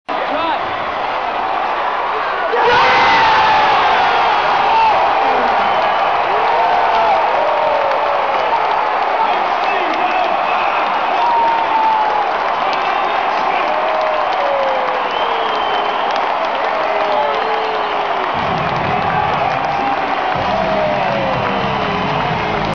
Crawford hits game winner over Pistons
Jamal Crawford hits the game winning shot to lead the Knicks over the Pistons on St. Patricks Day.  Knicks wore green and it worked.